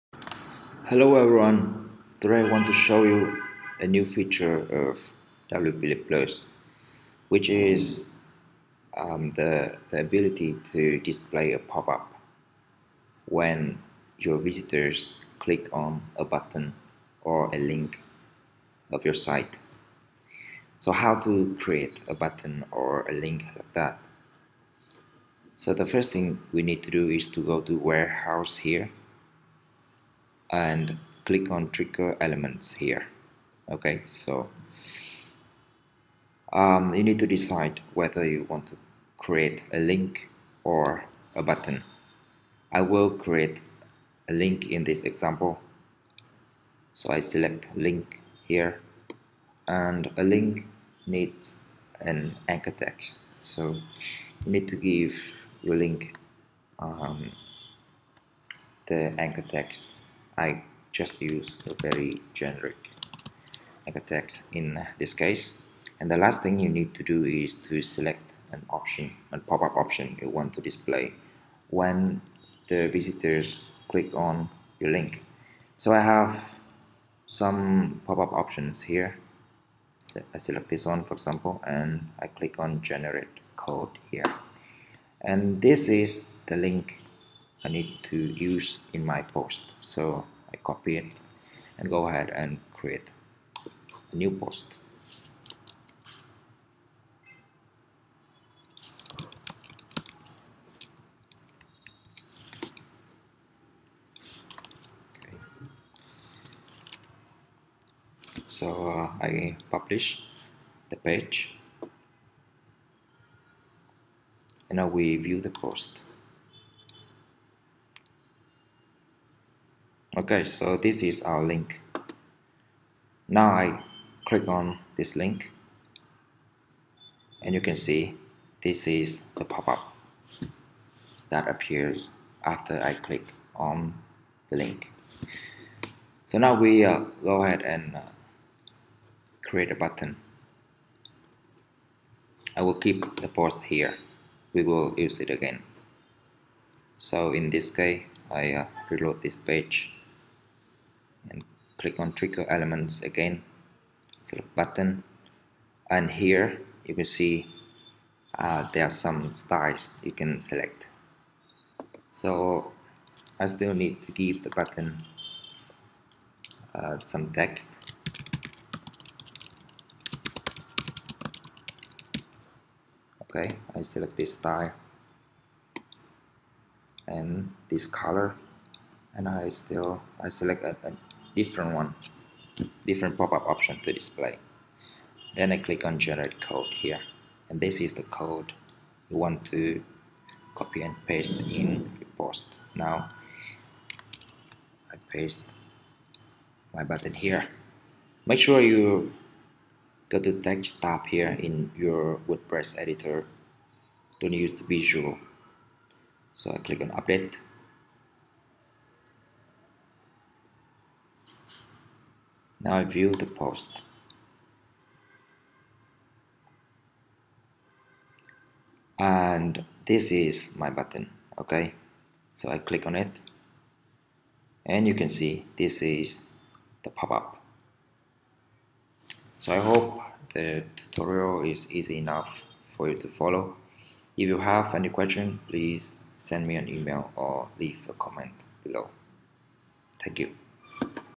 How To Create A Link Or A Button That Triggers Popup In WP Lead Plus
This video will show you how to create a link or a button that when your visitors click on it, a popup/widget will appear.

This is a feature of WP Lead Plus - A WordPress Plugin for generating leads.